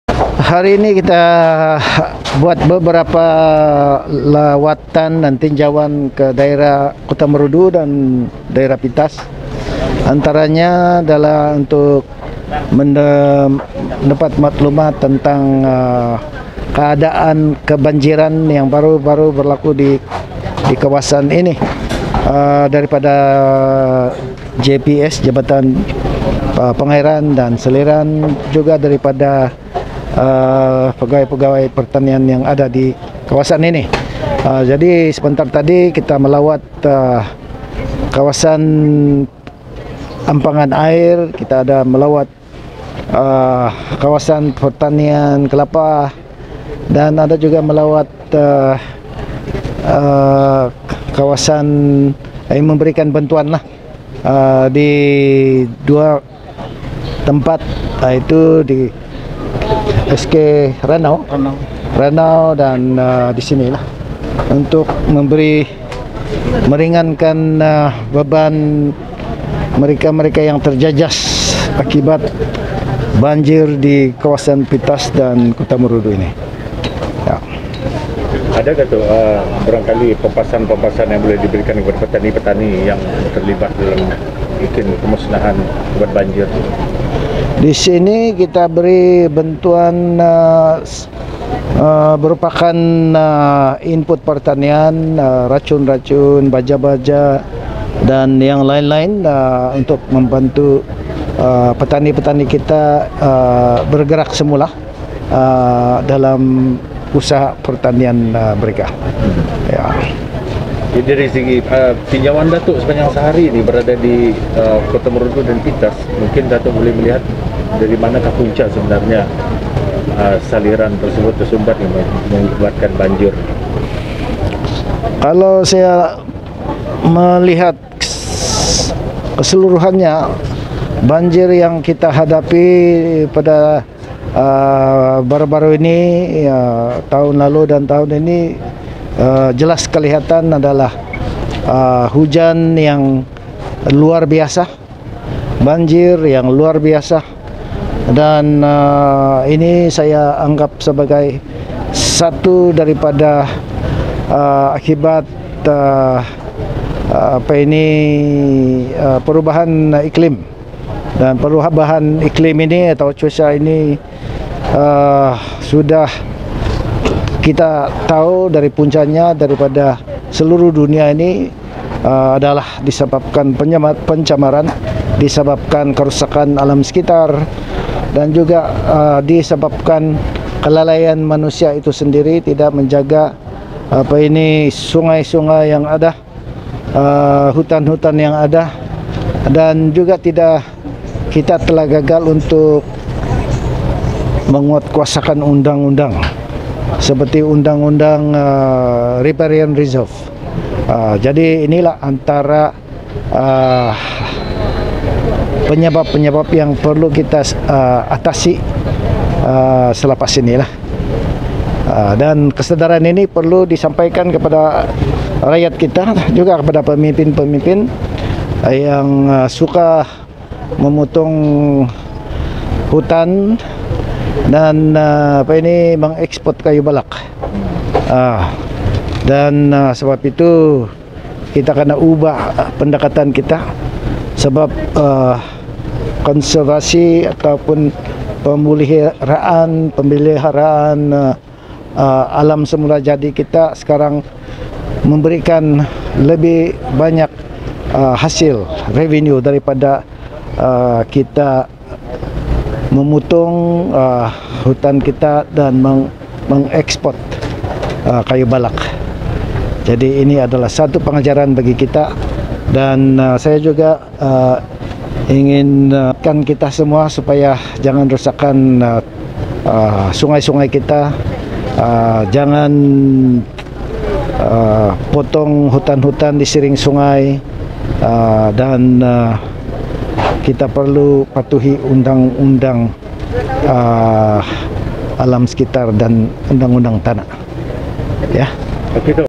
MARUDU & PITAS TERIMA LAWATAN RASMI TIMBALAN KETUA MENTERI SABAH
ISU BANJIR : Kota Marudu dan Pitas terima lawatan rasmi Timbalan Ketua Menteri merangkap Menteri Pertanian dan Perikanan Sabah YB DSP Dr Jeffrey Kitingan.